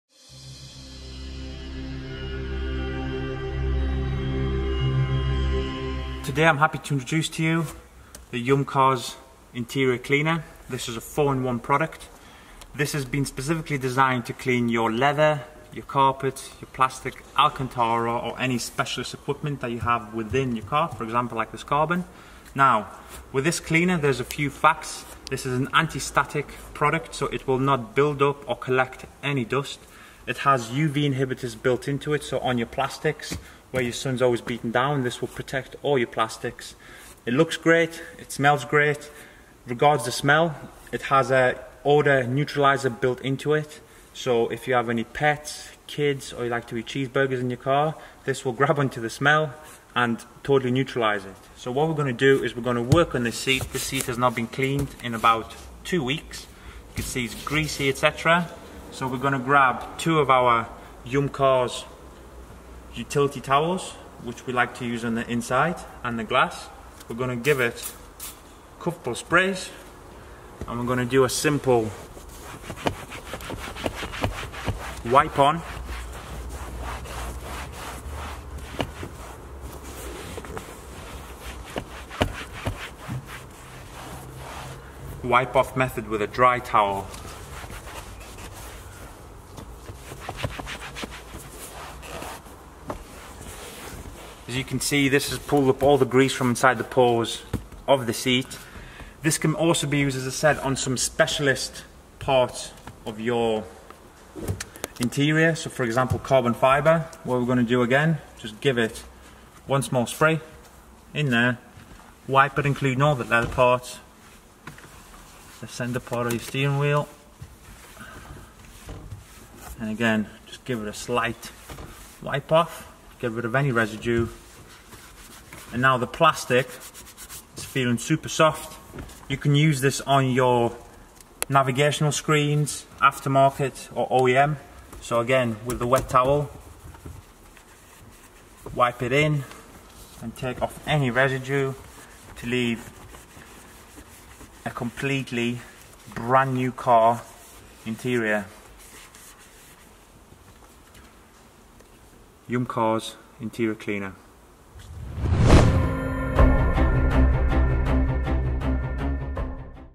YUM INTERIOR... HOW TO
As a professional detailing company when we tackle interiors you have to be prepared to potentially have 4 separate products. Leather / plastic / carpet / alcantara cleaners.

Now this can get really expensive and tiring to keep switching out chemicals for the multitude of finish on most modern cars.

This is where our interior cleaner comes in this can clean every interior component of the car apart from the glass, glass has a dedicated product. It will clean very gently whilst also not leaving any dressings or residues behind. 1 easy step for the whole inside detailing solution.